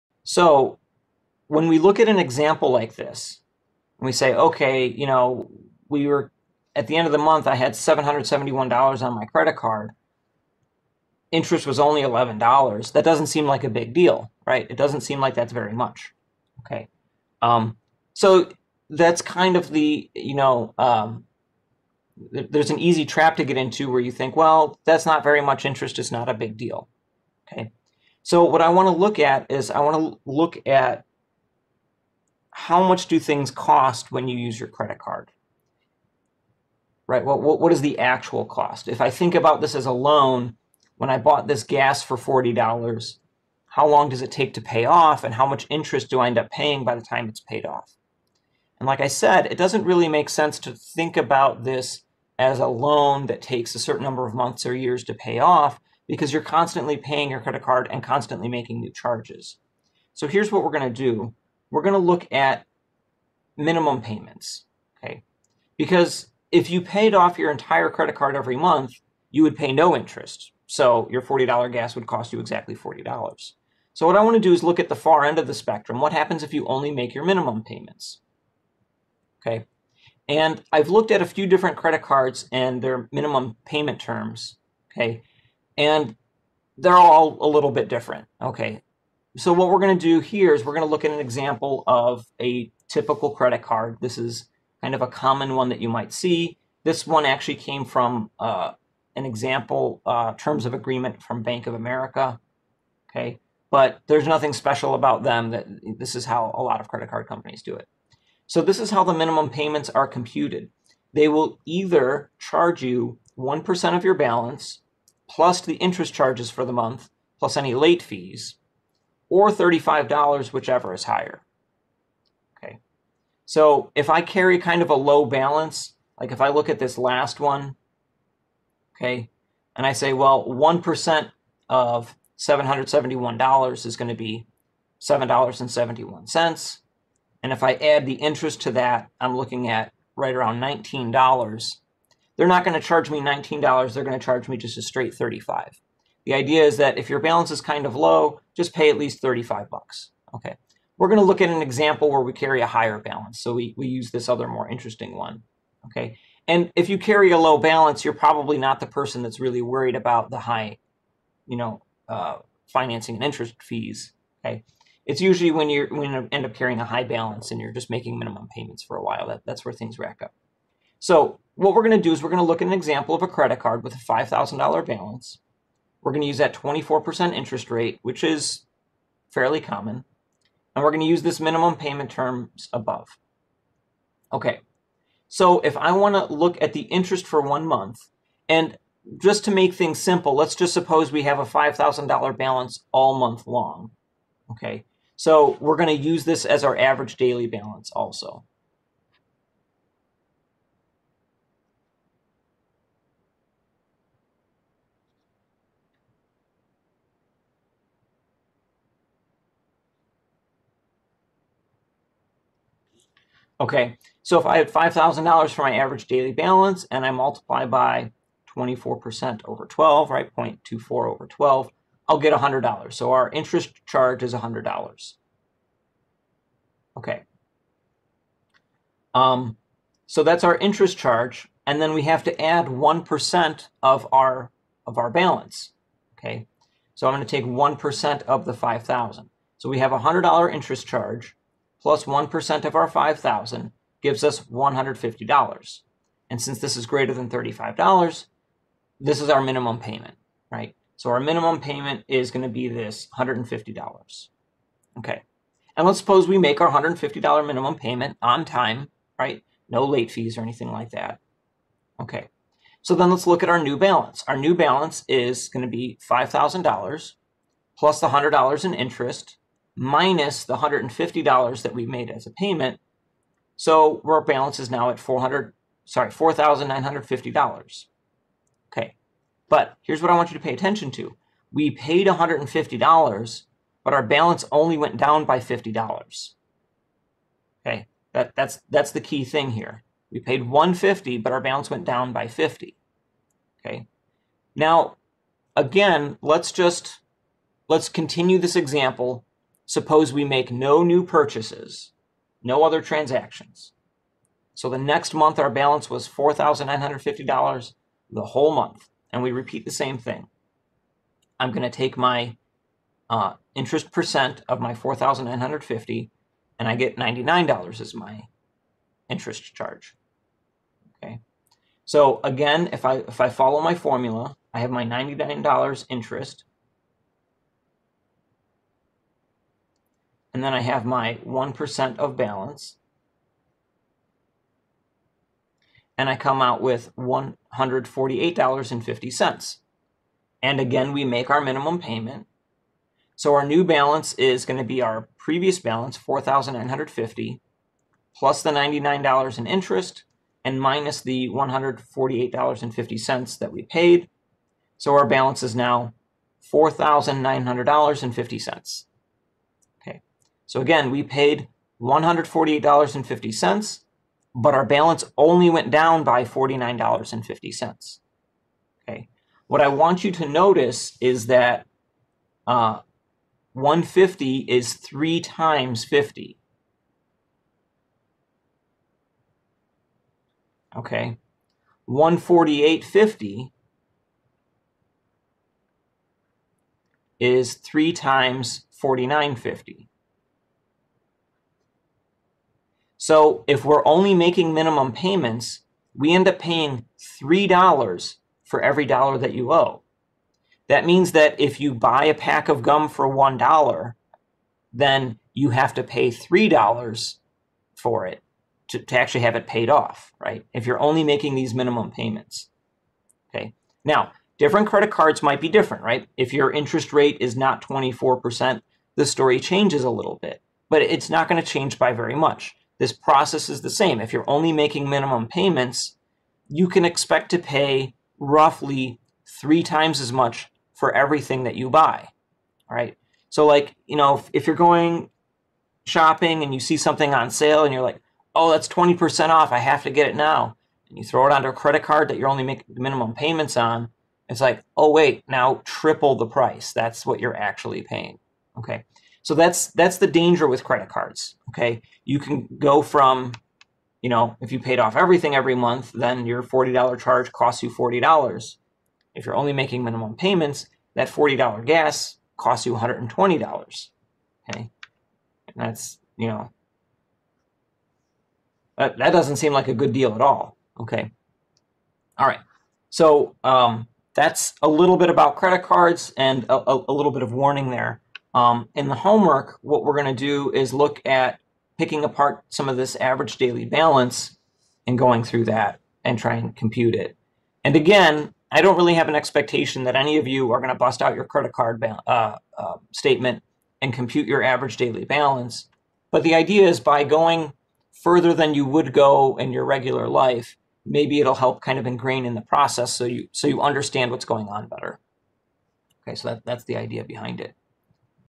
1.7.2 - Minimum Payments on a Credit Card
This example examines how much it costs to use a credit card when you only make the minimum payment.